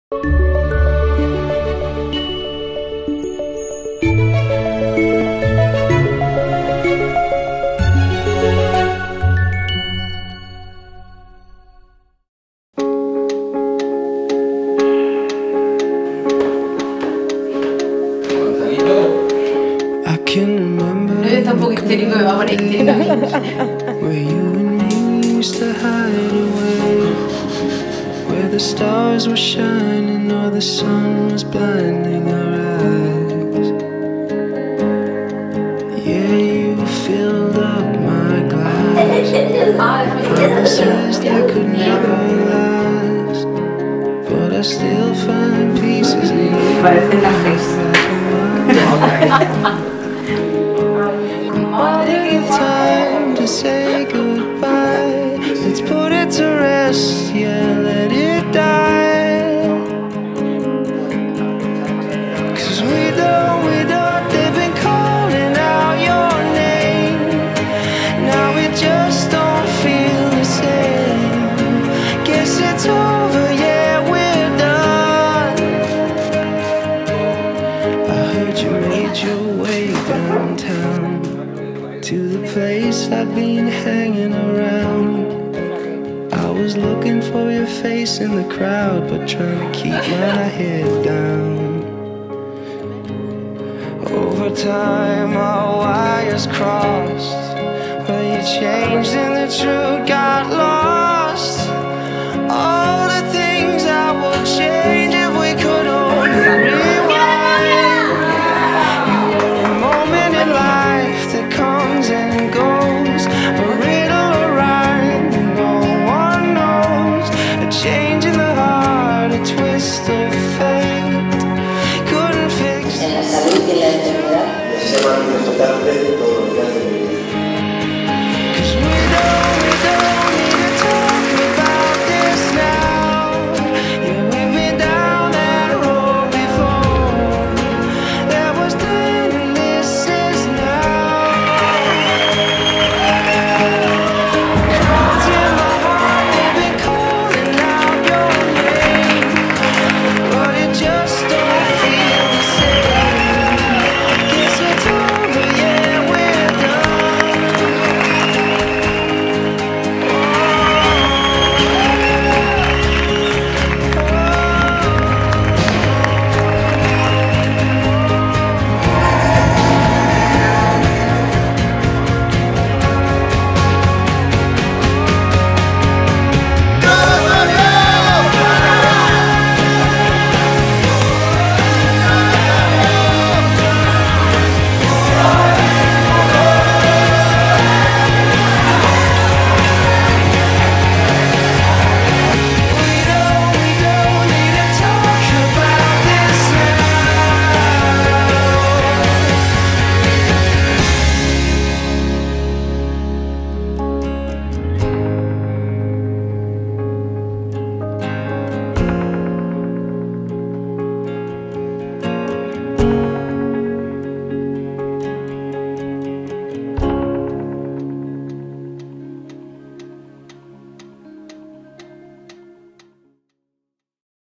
Trailer Pau e Irene. Vídeo Boda Murcia
Esta pareja se casó en la Iglesia del Balneario de Archena en Murcia y el convite se celebró en el Salón Pio XII (Jumilla). Y ya sabes, si te gusta da like y suscíbete. Gracias.